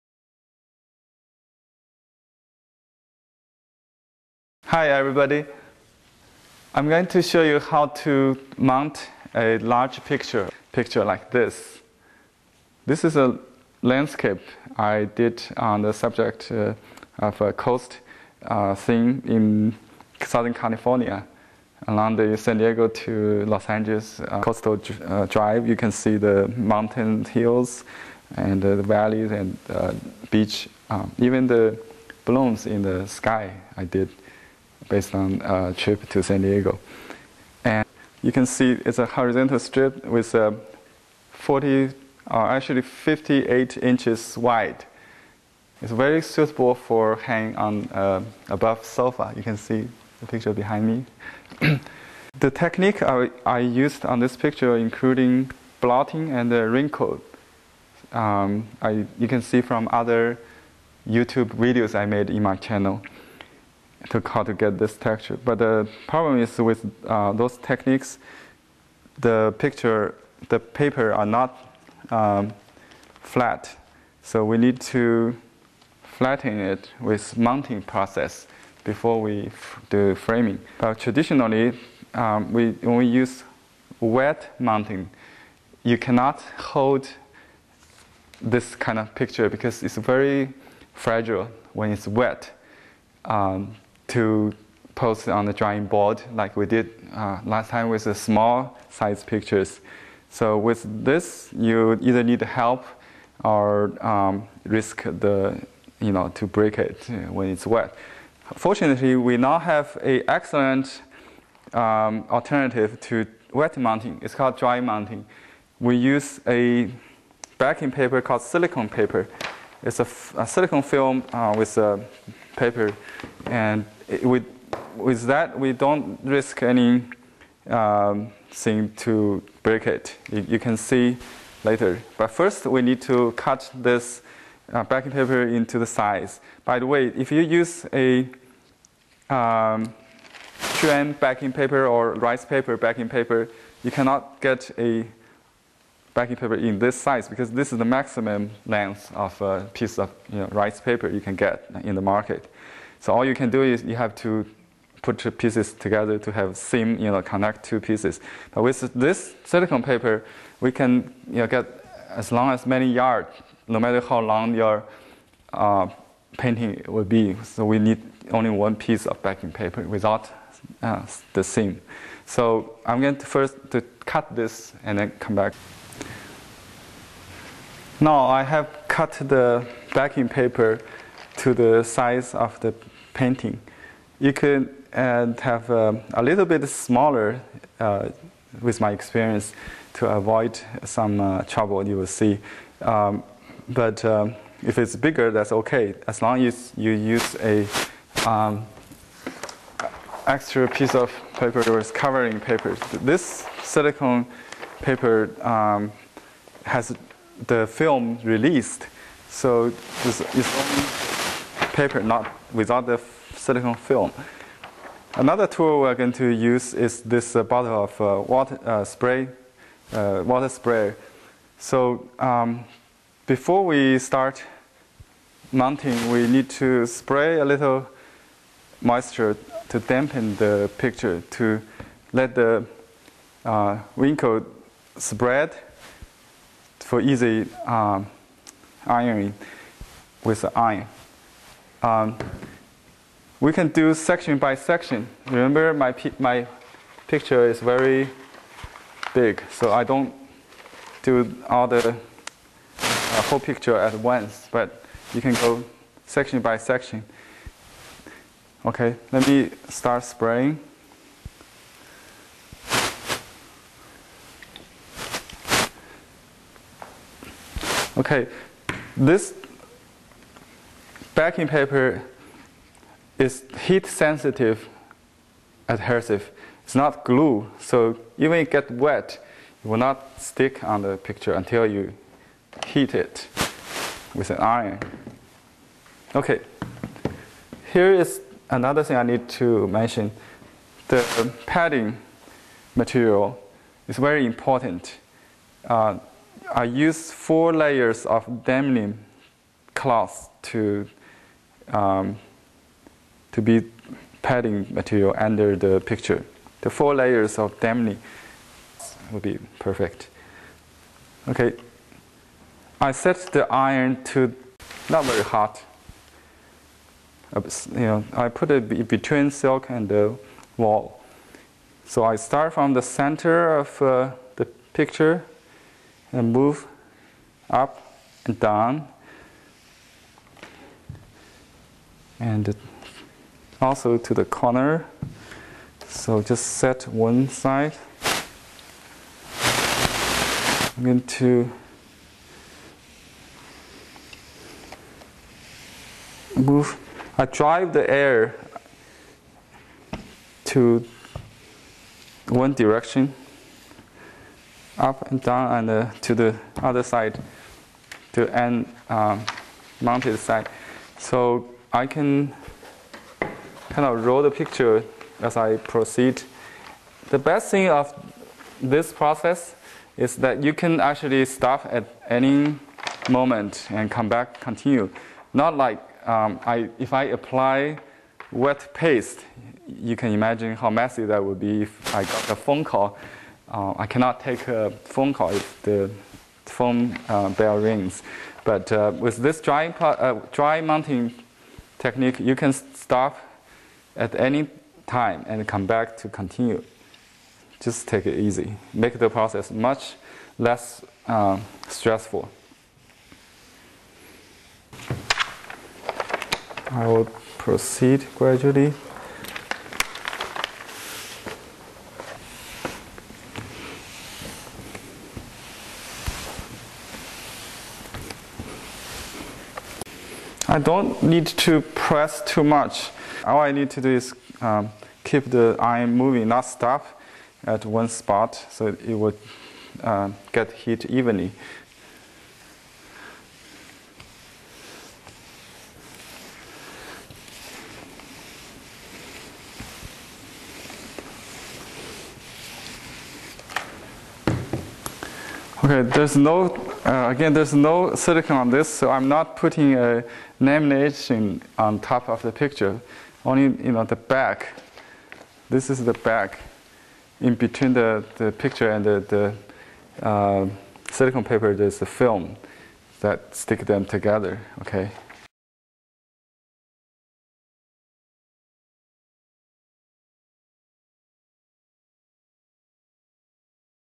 How to Mount a Large Chinese Painting (1)
To purchase Silicone Paper
How to dry-mounting large sized paintings on rice paper with Silicone Paper.